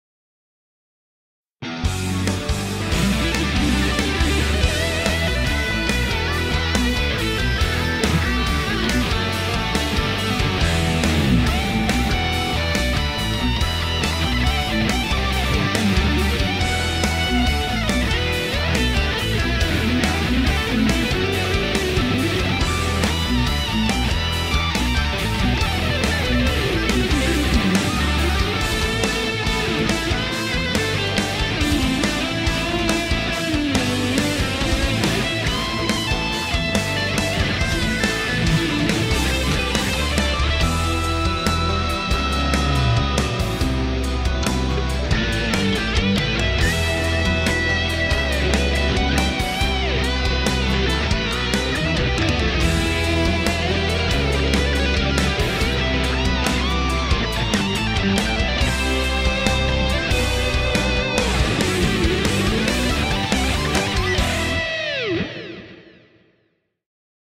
Ibanez Flying Fingers 2016 Dimitris Zafeiroudis,Veroia,GR
This is my entry for the Ibanez flying fingers competition.I'm playing with my Ibanez s5470 Prestige.Great instrument,always loved it and always will..!!!